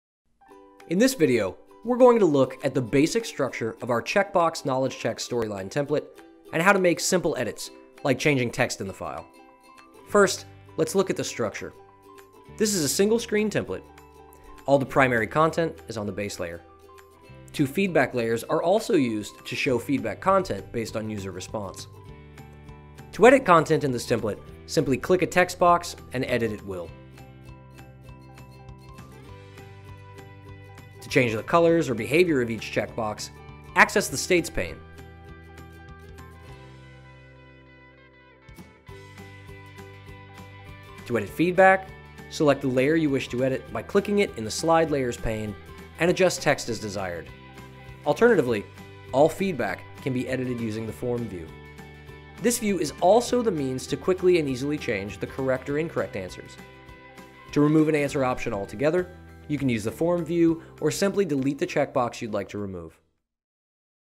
Storyline Styles: Check Box Knowledge Check
This video will help you understand how to edit the (Check Box Knowledge Check) template in our Storyline Styles.